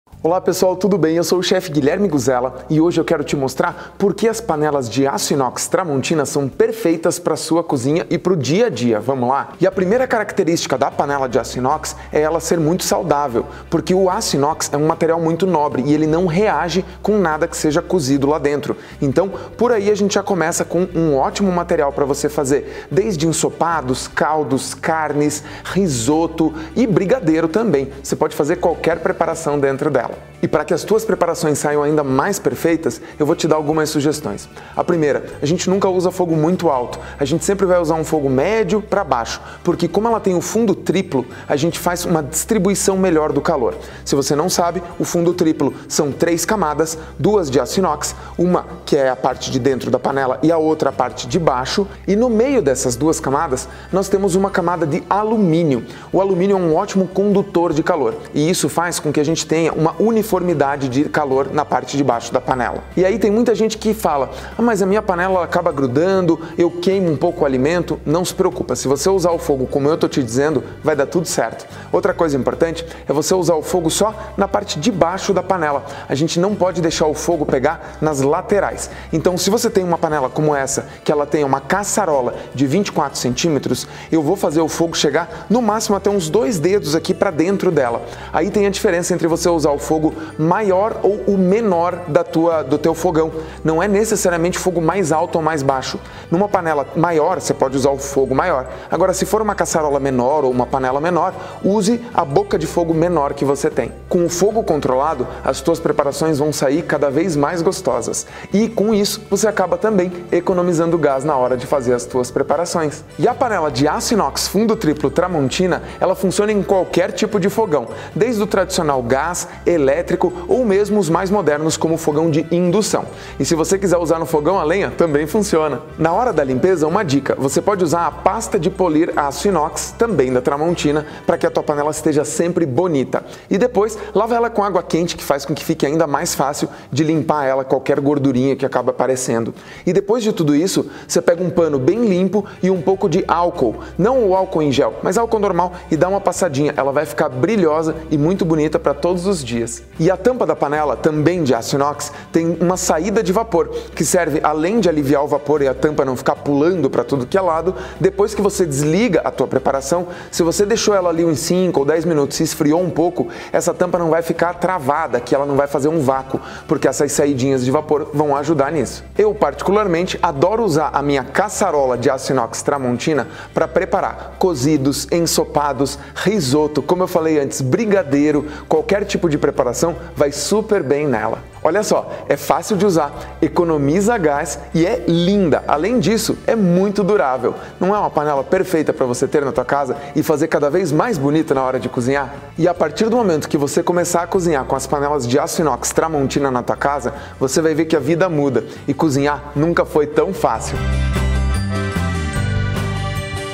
Caçarola Solar Aço Inox Tramontina | Lojas Colombo
Aprenda com o Chef Guilherme Guzela, o porquê as panelas de aço da Tramontina são ótimas para o seu dia a dia!

Para saber mais sobre outros produtos e serviços do Grupo Colombo, acesse os links: